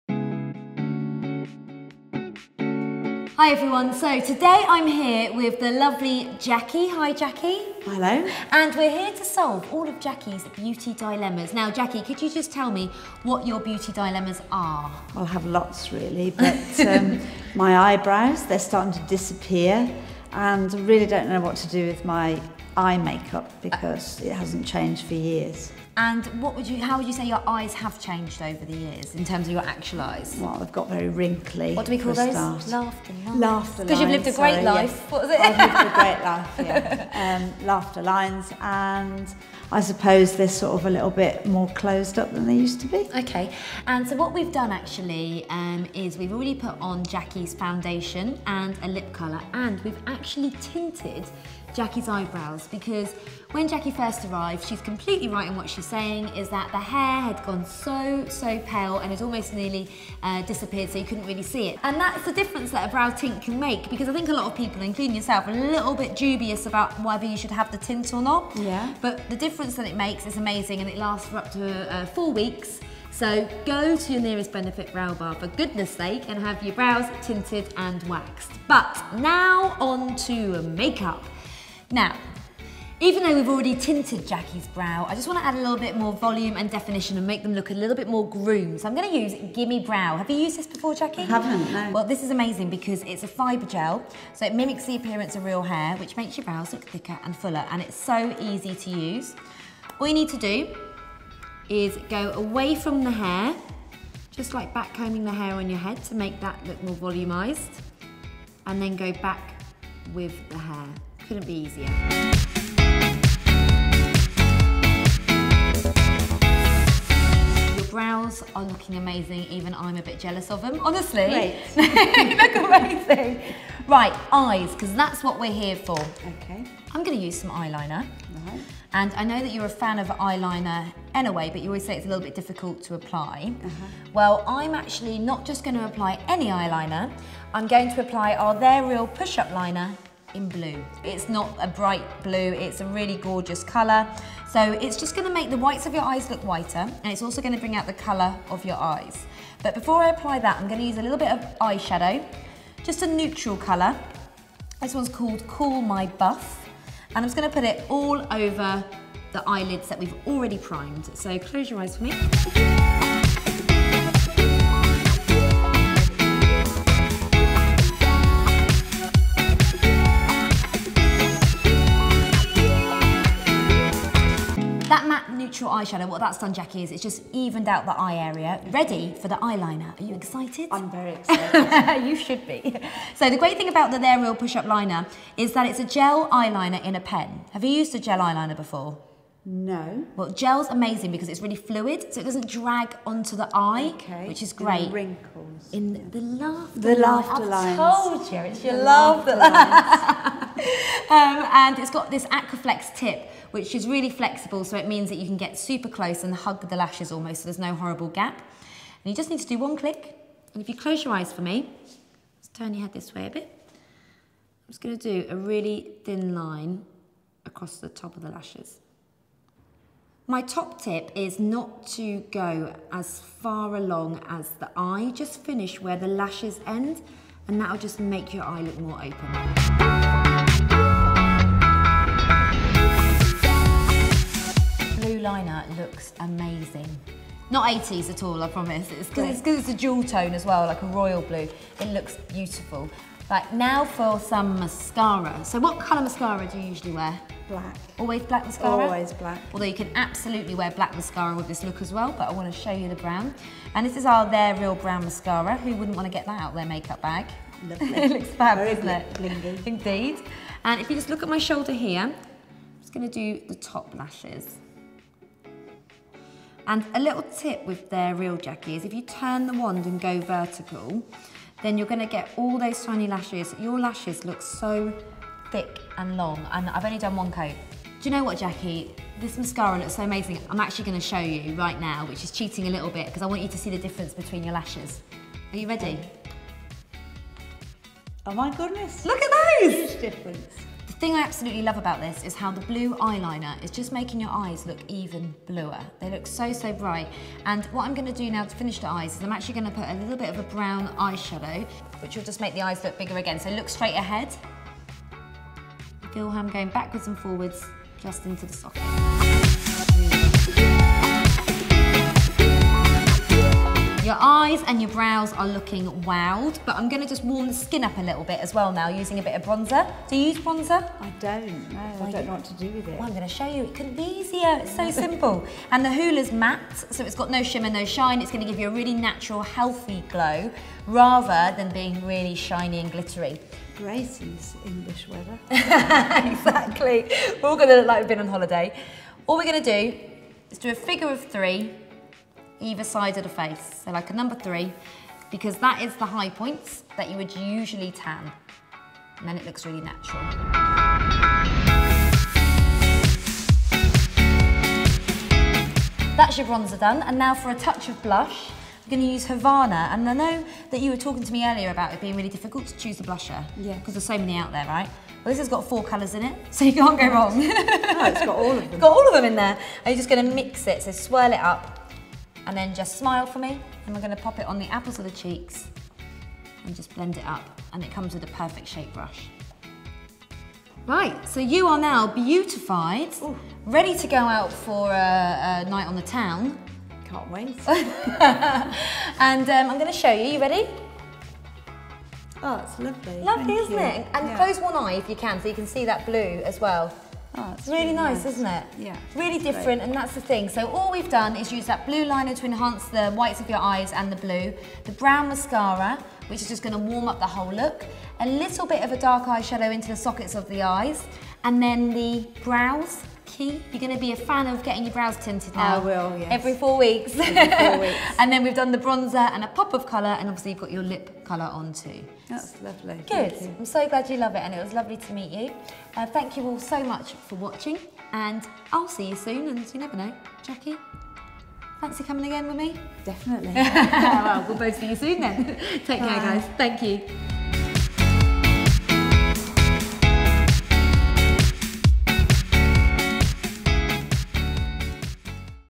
Make-up for over-50s: gorgeous eyes and thinning eyebrows
Benefit Cosmetics' Lisa Potter-Dixon meets High50 reader Jacqui and shows her how to get brighter, more open eyes, fuller brows and thicker lashes, and deal with laughter lines.